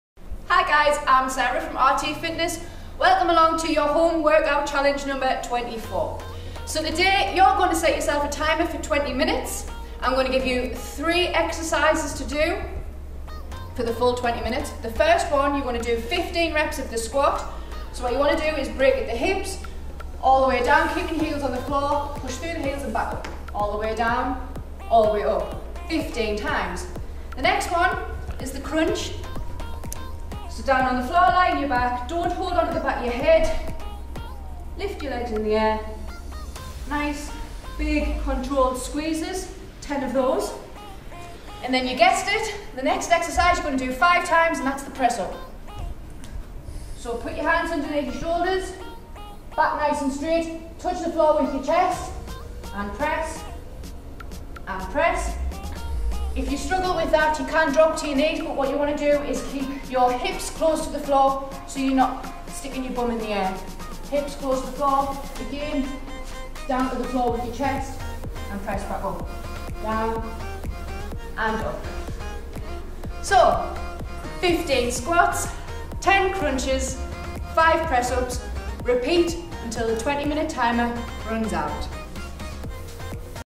Home workout challenge 24
Set timer for 20 mins

15 squats

10 crunch

5 press ups

Dont stop until the timer is complete